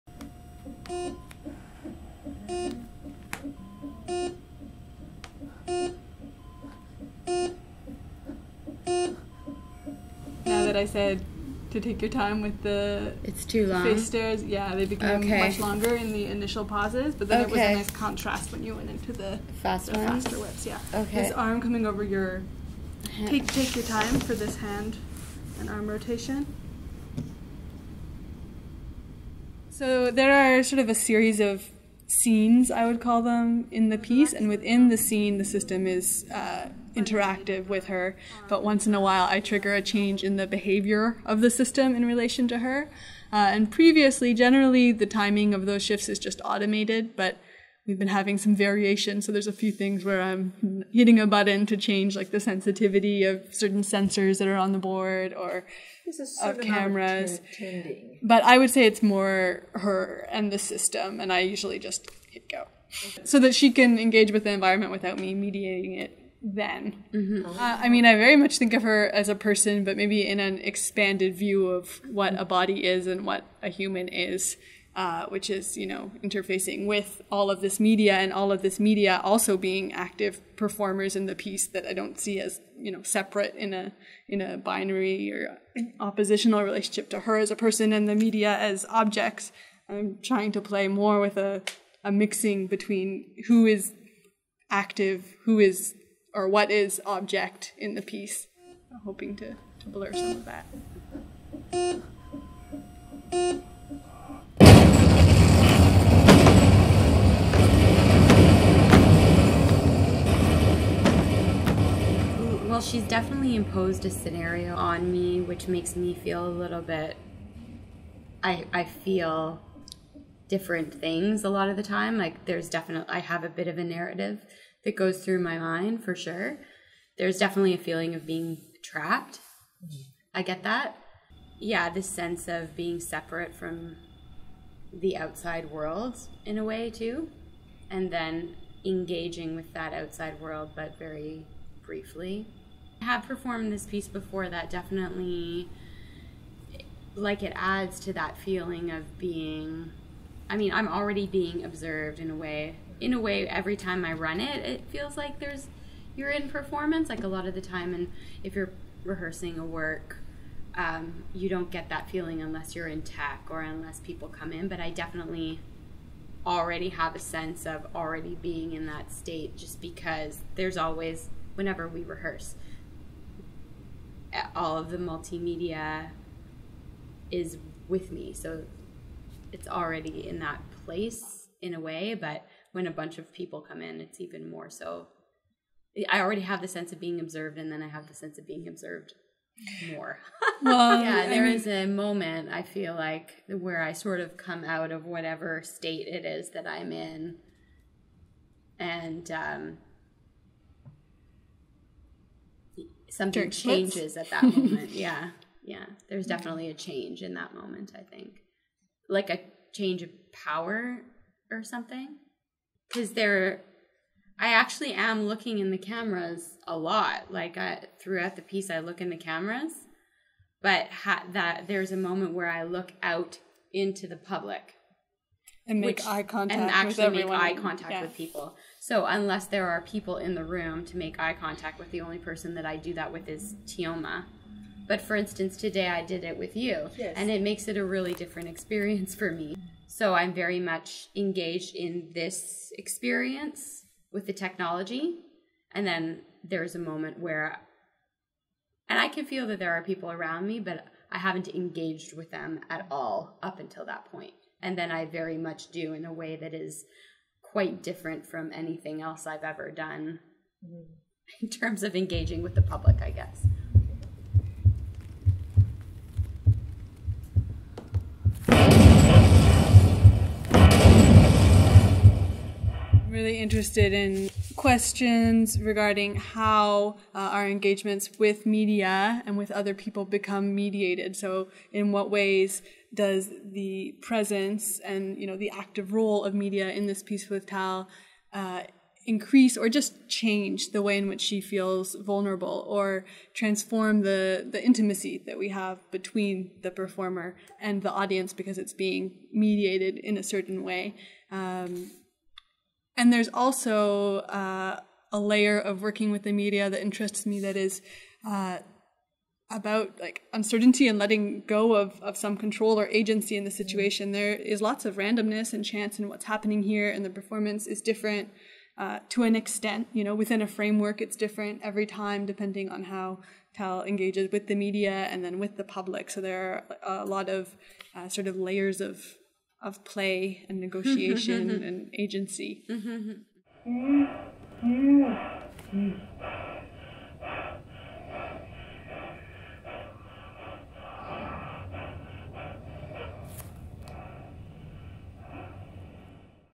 VISITE EN STUDIO | TEOMA NACCARATO | EXPERIENCE #1167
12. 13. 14 NOV. | 19H30
15 NOV. | 16H
Monument-National

Chorégraphe / Choreographers : Teoma Naccarato
Interprète / Performer : Tal Aronson